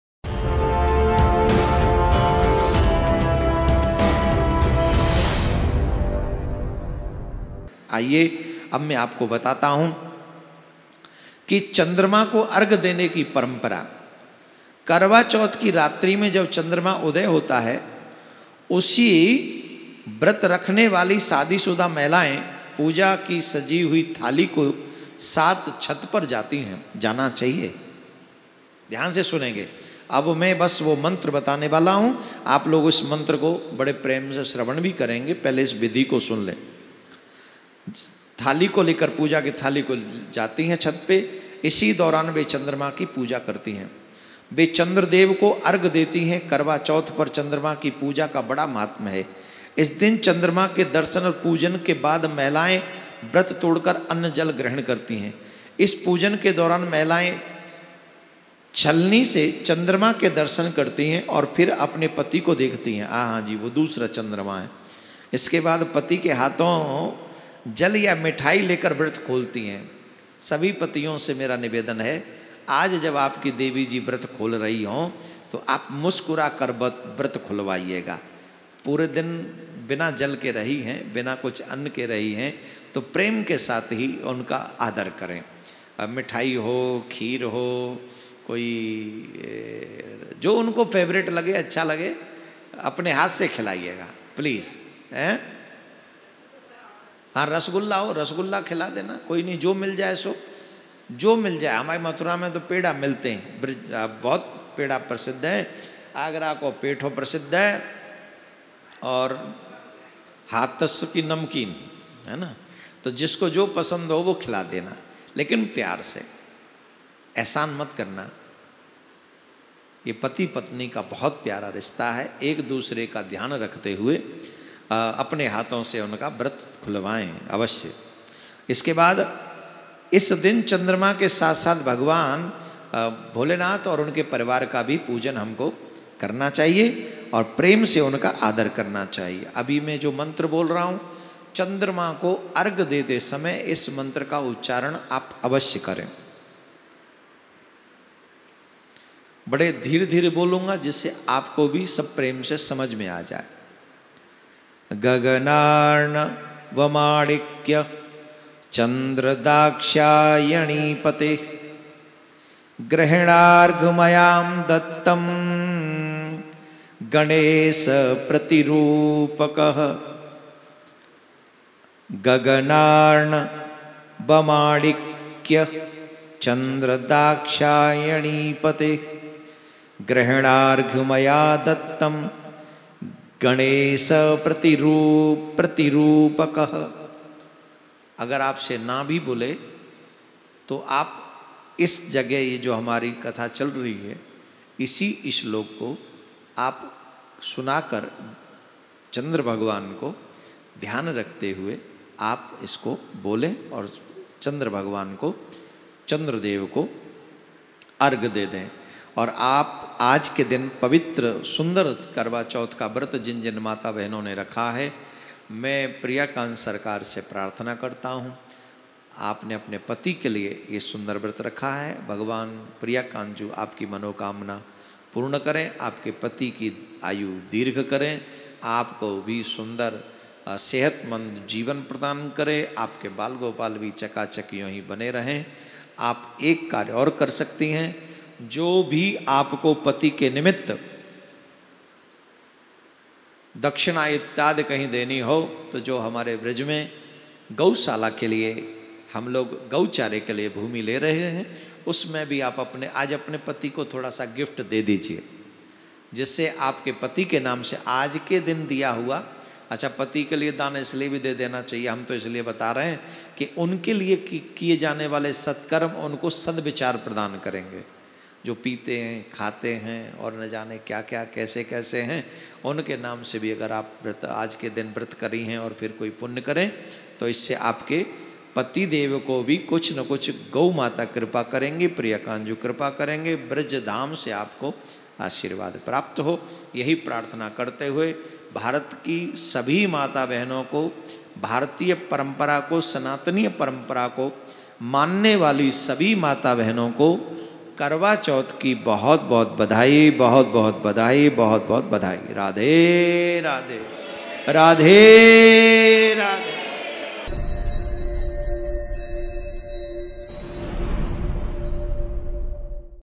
करवाचौथ पर इस विधि के साथ माता बहने चंद्र देव को अर्घ्य देकर करें व्रत पूरा || DEVKINANDAN THAKUR JI
करवाचौथ पर इस विधि के साथ माता बहने चंद्र देव को अर्घ्य देकर करें व्रत पूरा || SHRI DEVKINANDAN THAKUR JI

Copyright : DevkinandanJiMaharaj